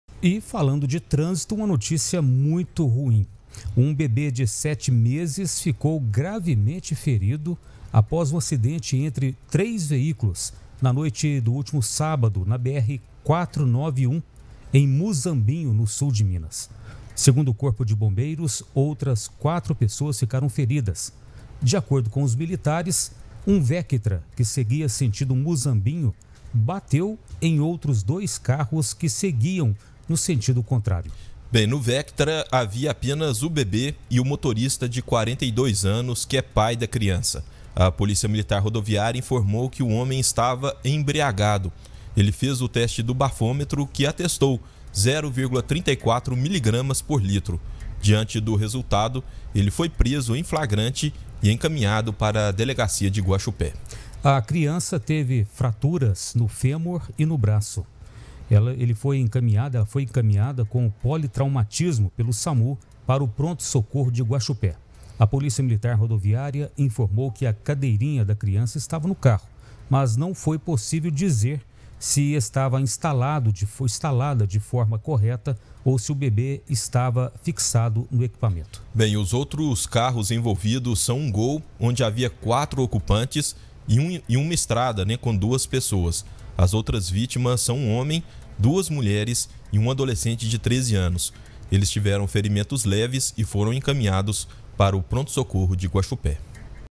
PAI EMBRIAGADO DIRIGE CARRO, CAUSA ACIDENTE E FILHO DE 7 MESES FICA GRAVEMENTE FERIDO
| REGIÃO |

Um bebê de sete meses ficou gravemente ferido após um acidente entre três veículos na noite do último sábado, na BR-491, em Muzambinho, no sul de Minas. Saiba mais informações sobre esse caso: